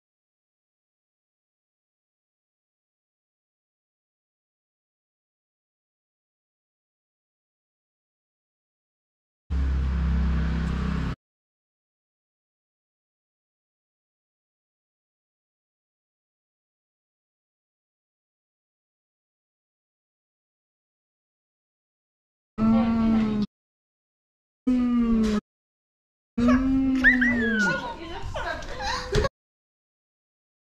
Gopro Hero 10 test at night warp and normal mixed
Video from James Lalrinawma